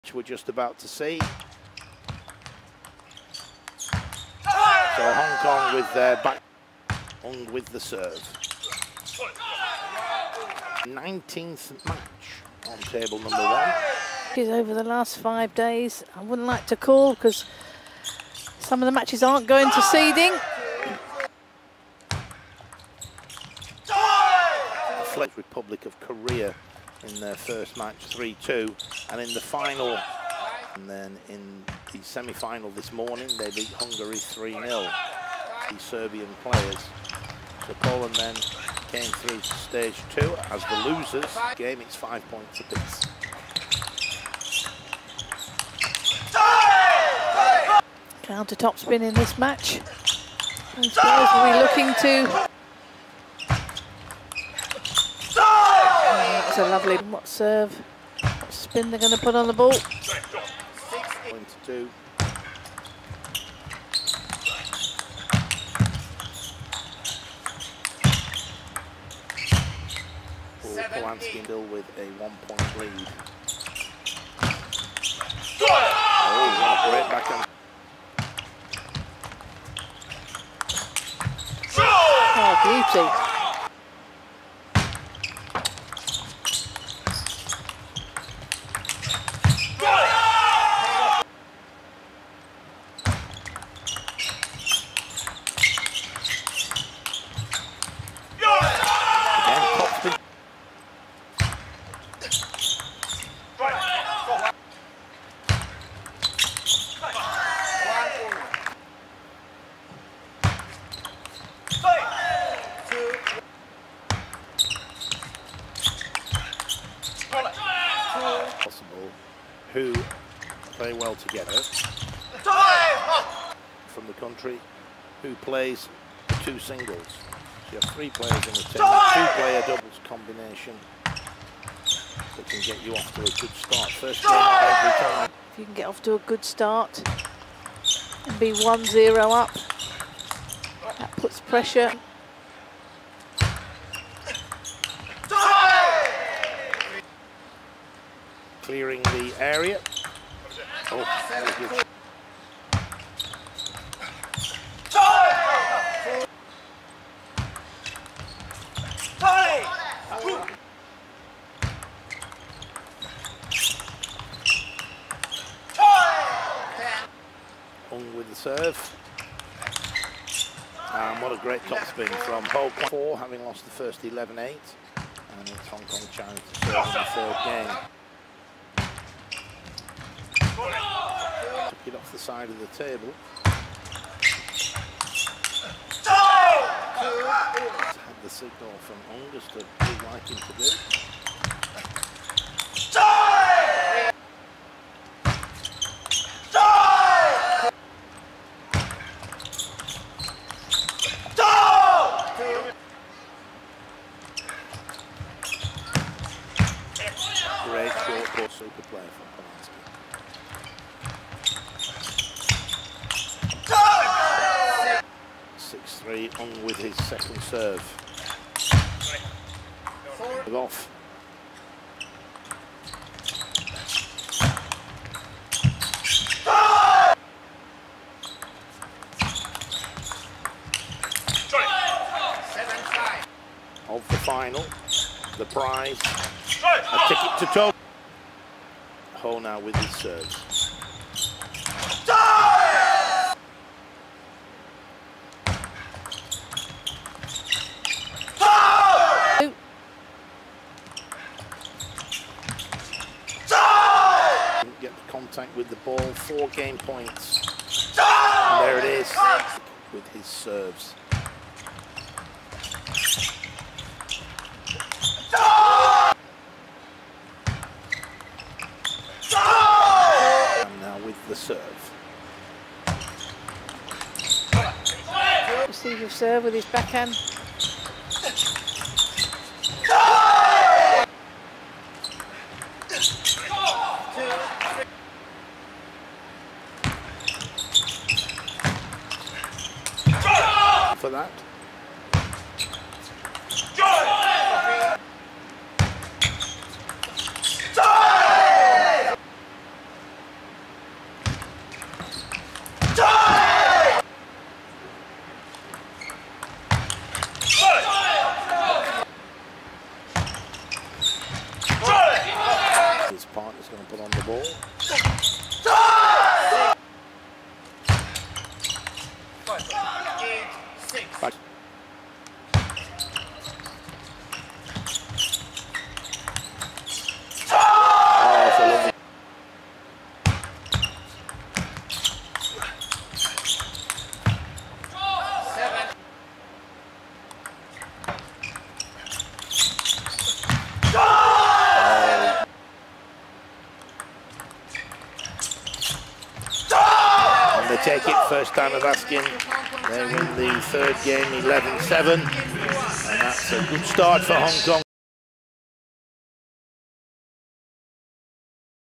Ho Kwan Kit/Ng Pak Nam vs Lubomir Jancarik/Tomas Polansky | 2020 World Team Qualification (Final)
Review all the highlights from the Ho Kwan Kit/Ng Pak Nam vs Lubomir Jancarik/Tomas Polansky at the 2020 ITTF World Team Qualification Tournament.